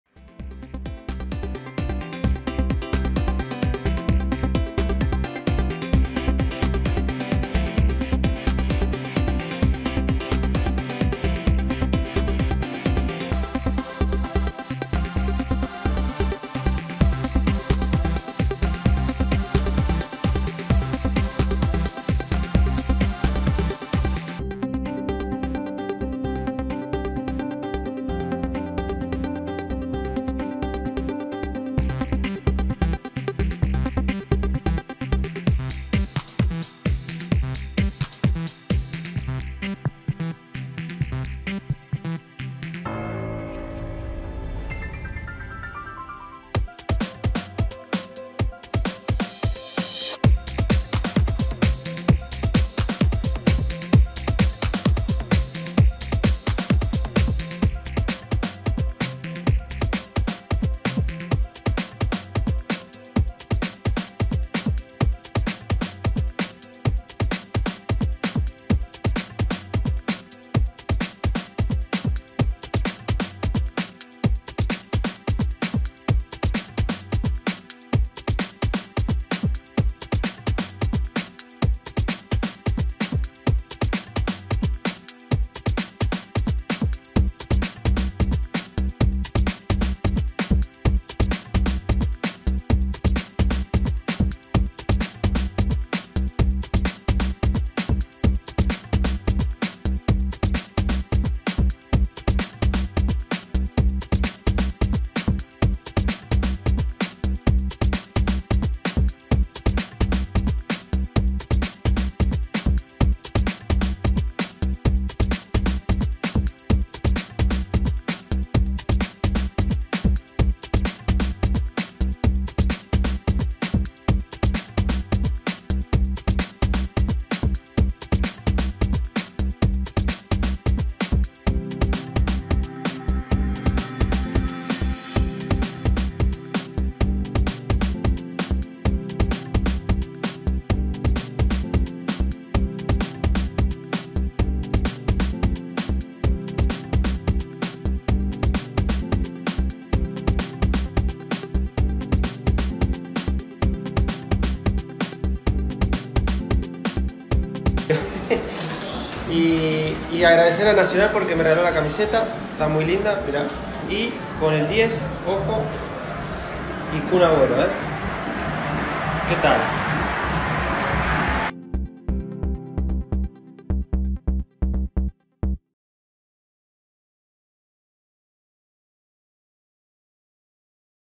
سواريز يوبخ أجويرو بسبب "ناسيونال".. ولاعب سيتي يقدم لفتة طيبة
قام سيرخيو أجويرو لاعب برشلونة ومانشستر سيتي السابق بلفتة طيبة تجاه لويس سواريز لاعب برشلونة وأتلتيكو مدريد السابق وناسيونال الحالي، بعدما حضر مباراته الأولى ذهاب ربع نهائي كأس أمريكا الجنوبية 2022